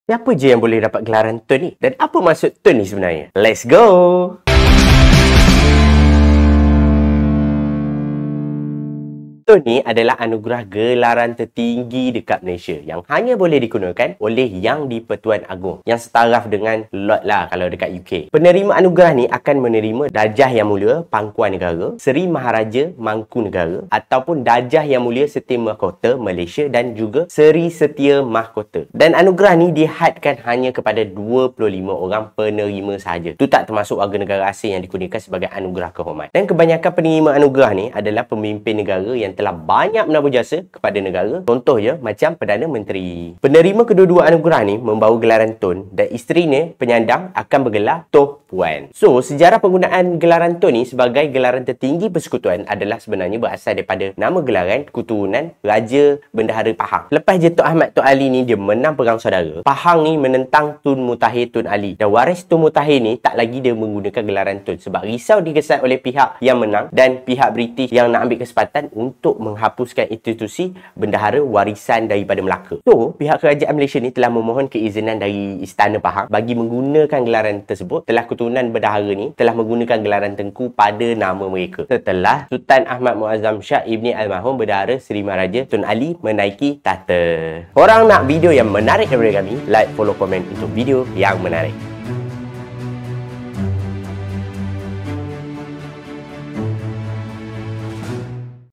Siapa Je Boleh Dapat Gelaran TUN?
Tun ni adalah anugerah gelaran tertinggi dekat Malaysia yang hanya boleh dikurniakan oleh Yang di-Pertuan Agong yang setaraf dengan gelaran "Lord" di United Kingdom.